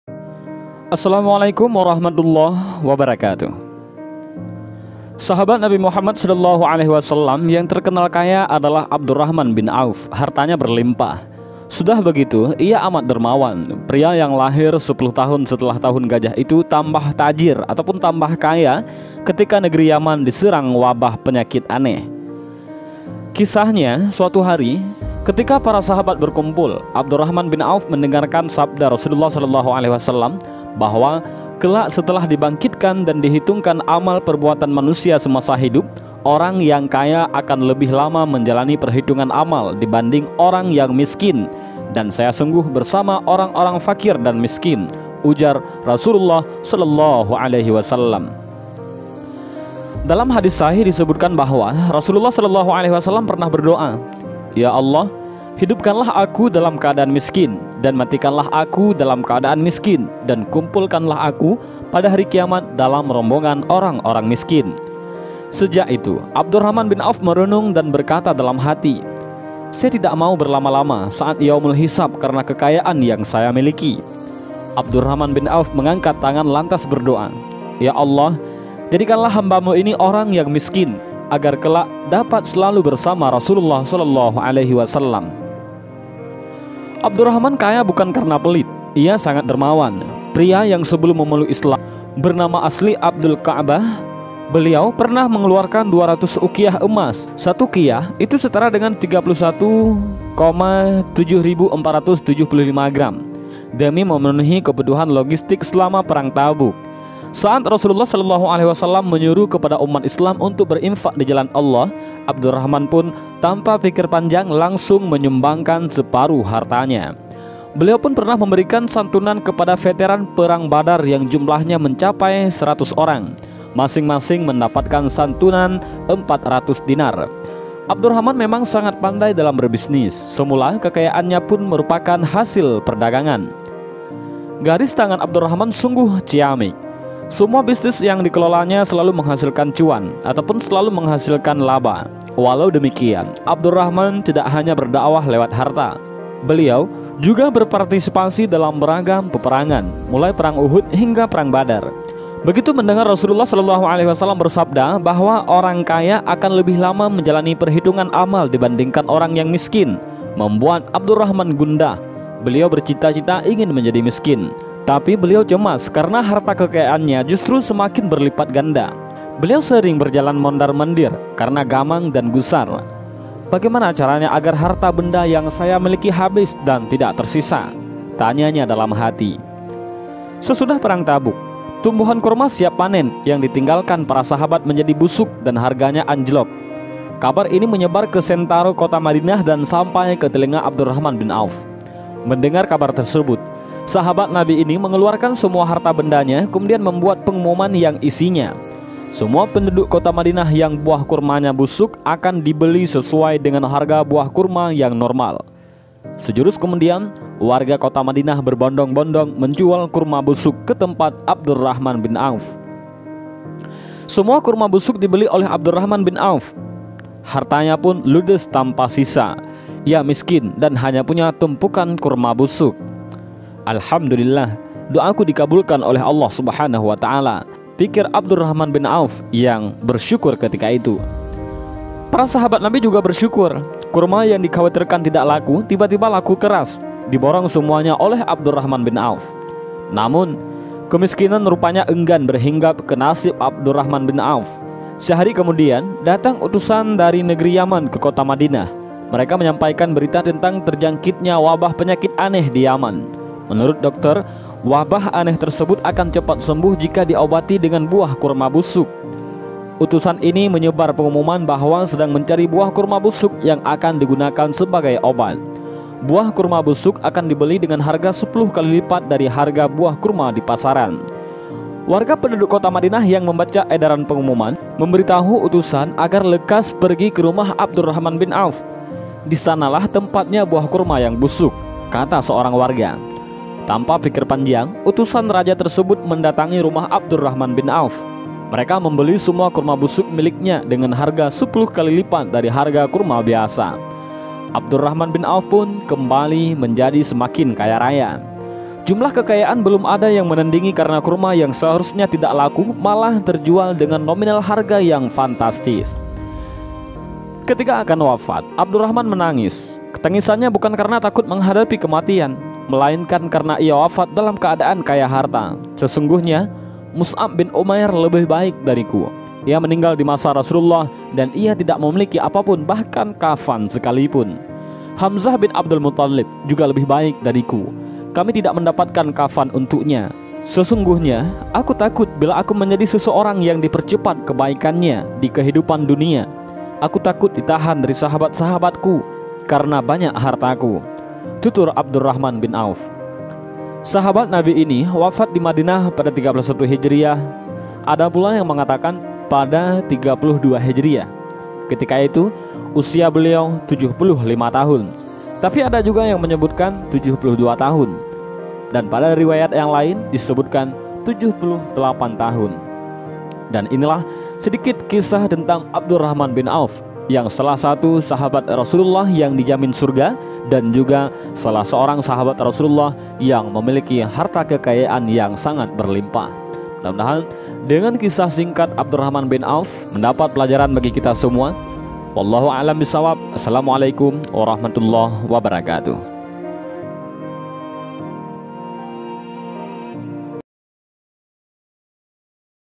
Berdoa Agar Miskin, Sahabat Nabi Ini Malah Tambah Kaya Raya. Abdurrahman Bin Auf
Abdurrahman bin Auf Bertambah Kaya Raya karena Kurma Busuk
SAHABAT Nabi Muhammad shallallahu 'alaihi wasallam (SAW) yang terkenal kaya adalah Abdurrahman bin Auf. Hartanya berlimpah. Sudah begitu ia amat dermawan. Pria yang lahir 10 tahun setelah Tahun Gajah itu tambah tajir ketika Negeri Yaman diserang wabah penyakit aneh.

Ceritanya begini. Suatu hari, ketika para sahabat berkumpul, Abdurrahman bin Auf mendengarkan sabda Rasulullah bahwa kelak setelah dibangkitkan dan dihitungnya amal perbuatan manusia semasa hidup, "orang yang kaya akan lebih lama menjalani perhitungan amal dibanding orang yang miskin dan saya sungguh bersama orang-orang fakir dan miskin," ujar Rasulullah.